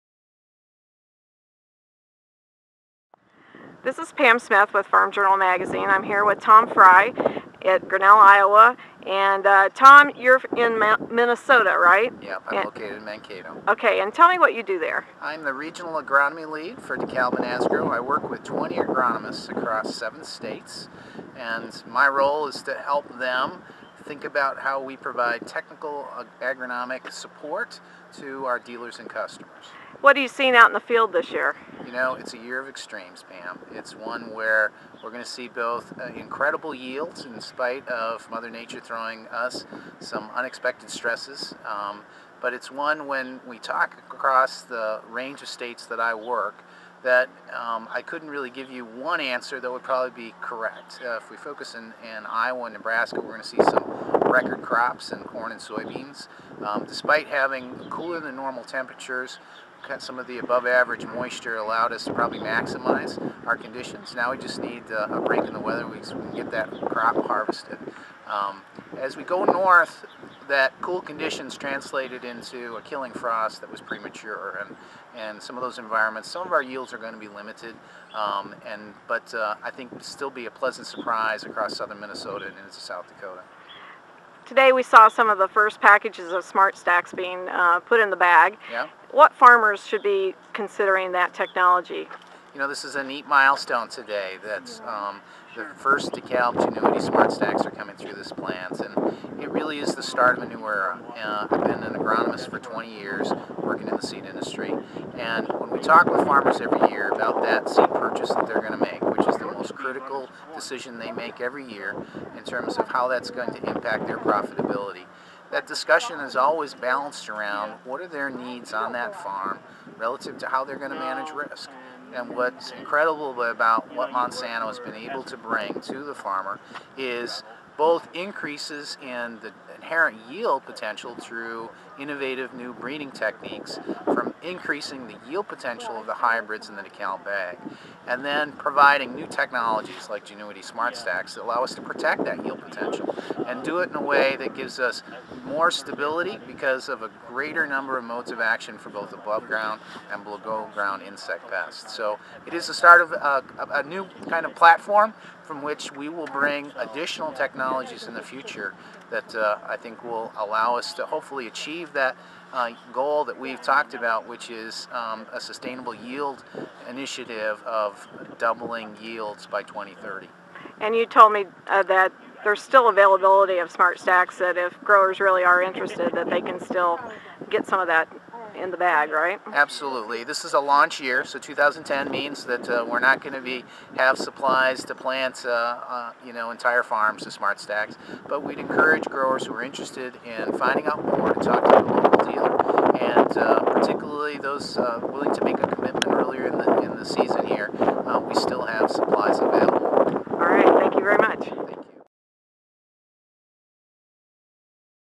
Farm Journal: Uses for Monsantos SmartStax
Tom Fry, Monsanto region agronomy lead, discusses the condition of the corn crop in his area and where SmartStax fits.